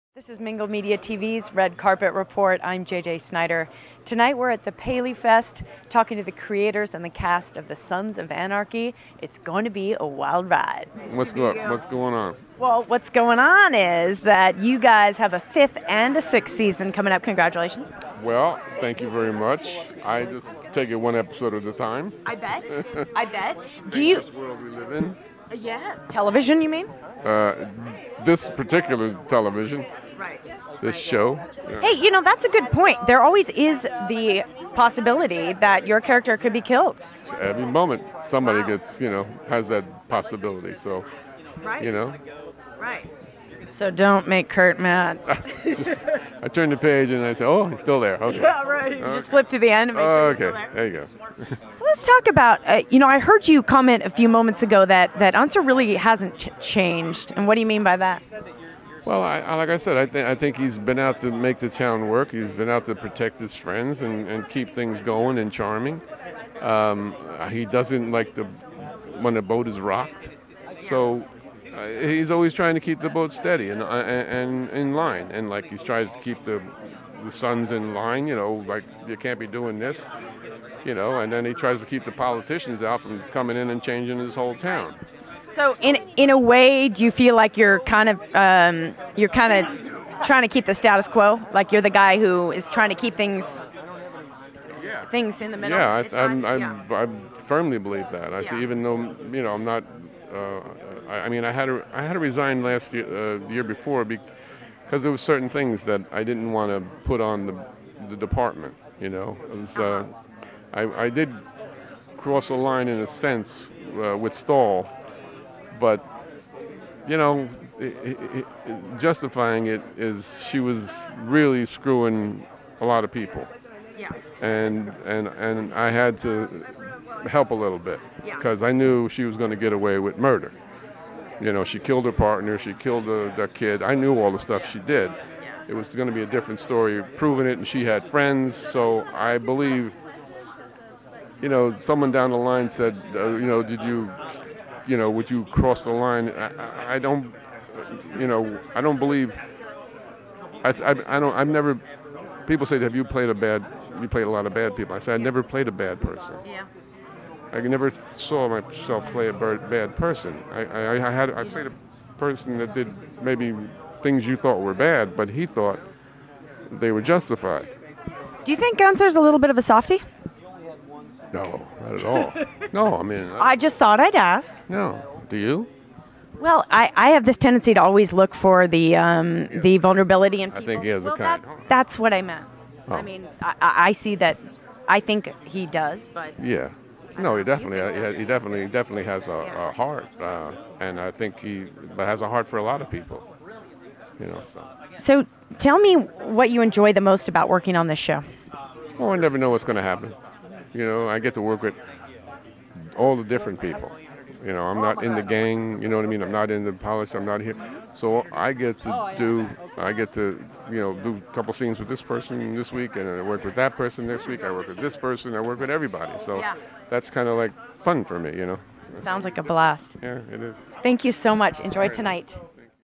Dayton Callie at Sons of Anarchy: PaleyFest 2012 Red Carpet
Mingle Media TV were invited to come out and cover the Sons of Anarchy night at the PaleyFest 2012 from the Red Carpet at the Saban Theatre. The annual William S. Paley Television Festival celebrates television's rich and diverse programming and the creative process behind the medium since 1984.

Cast members of the drama "Sons of Anarchy" on FX were in attendance as were creator Kurt Sutter, creator/producer to talk about their first four seasons and gave some hints about the upcoming season five at PaleyFest on Wednesday. They will starting shooting its fifth season in May and will focus on Jax.